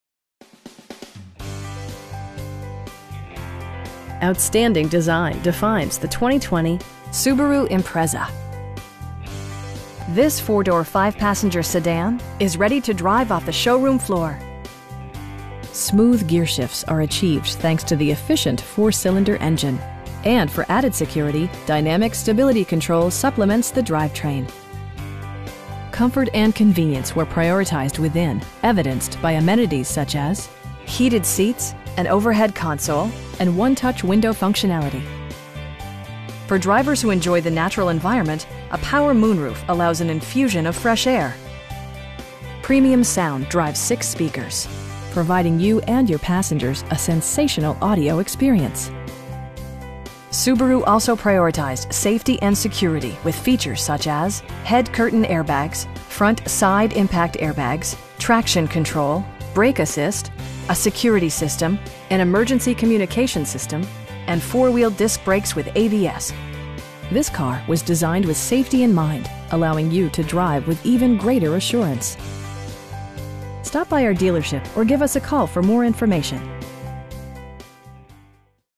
2020 Subaru Impreza Premium in Alexandria, VA 22306
Beyer Subaru
7416 Richmond HWY

Outstanding design defines the 2020 Subaru Impreza. This 4 door, 5 passenger sedan is ready to drive off the showroom floor! Smooth gearshifts are achieved thanks to the efficient 4 cylinder engine, and for added security, dynamic Stability Control supplements the drivetrain. Comfort and convenience were prioritized within, evidenced by amenities such as: heated seats, telescoping steering wheel, power door mirrors, power windows, cruise control, an overhead console, and 1-touch window functionality. For drivers who enjoy the natural environment, a power moon roof allows an infusion of fresh air. Premium sound drives 6 speakers, providing you and your passengers a sensational audio experience. Subaru also prioritized safety and security with features such as: head curtain airbags, front side impact airbags, traction control, brake assist, a security system, an emergency communication system, and 4 wheel disc brakes with ABS. This car was designed with safety in mind, allowing you to drive with even greater assurance. We pride ourselves in the quality that we offer on all of our vehicles. Stop by our dealership or give us a call for more information.